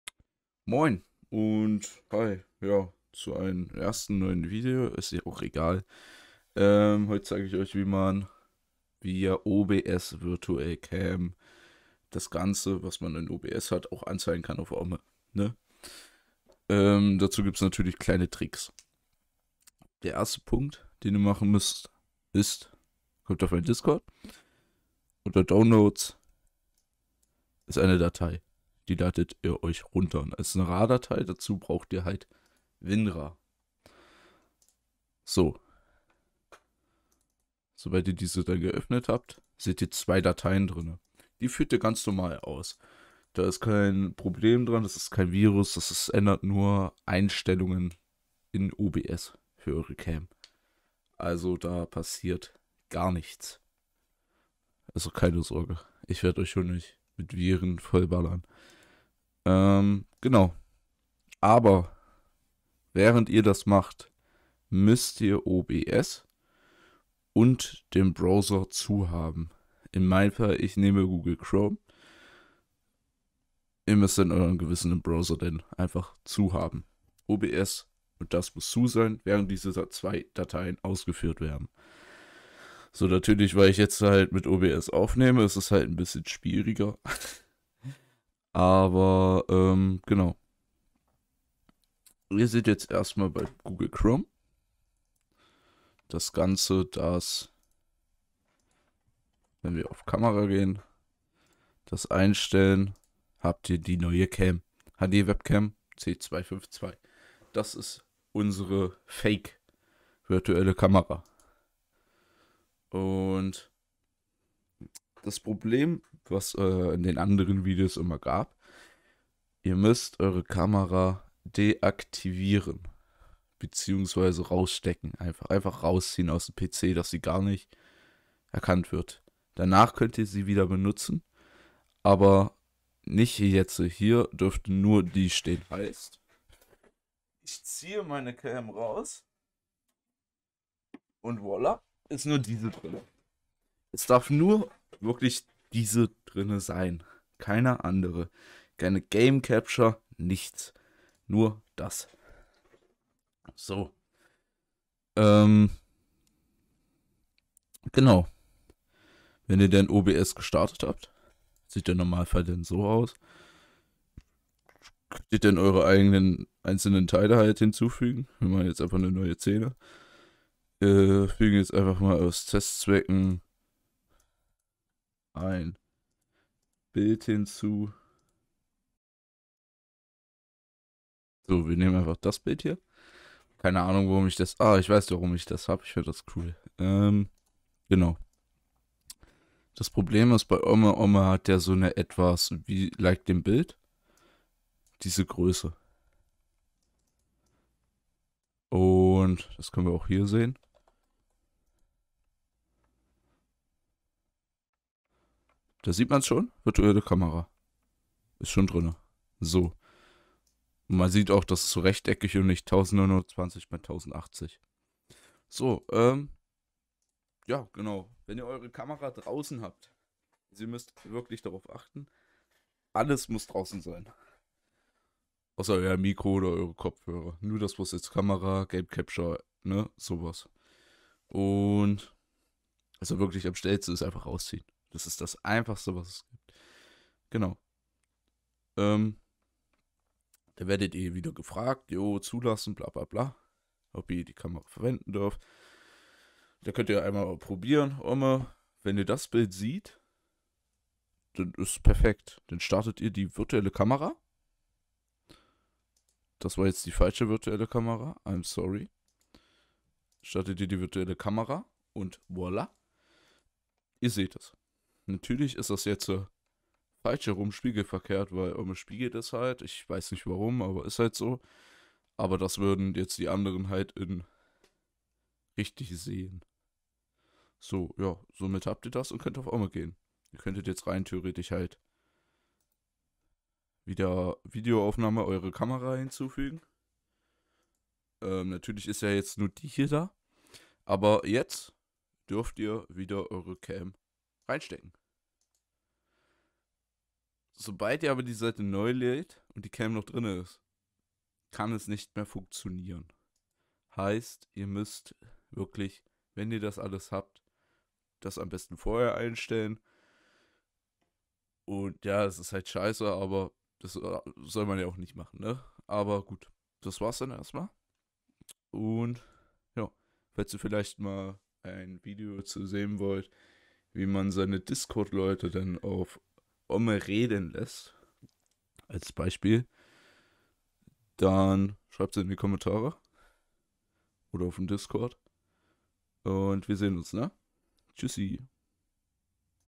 OBS Virtuelle Kamera auf OmeTV benutzen! | Deutsch
In diesen Video wird euch erklärt wie ihr eure Virtuelle Kamera auf OmeTV benutzen könnt!

Lasst gerne ein Abo und Like da :)